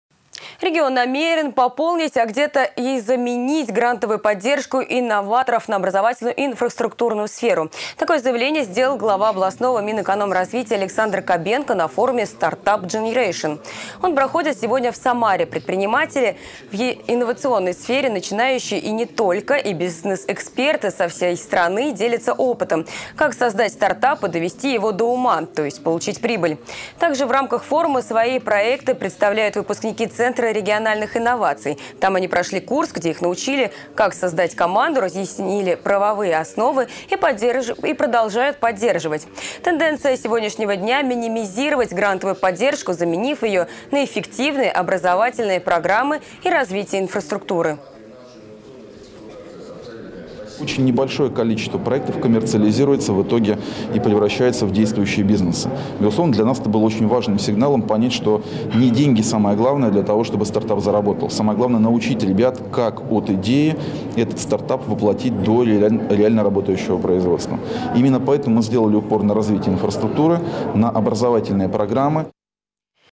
В Самаре проходит форум «Startup Generation 2.0: от идеи к инвестициям»
Регион намерен дополнить, а где-то и заменить грантовую поддержку инноваторов на образовательную и инфраструктурную. Такое заявление сделал глава областного Минэкономразвития Александр Кобенко на форуме «Startup Generation 2.0: от идеи к инвестициям» . Он проходит сегодня в Самаре. Предприниматели в инновационной сфере и бизнес-эксперты со всей страны делятся опытом.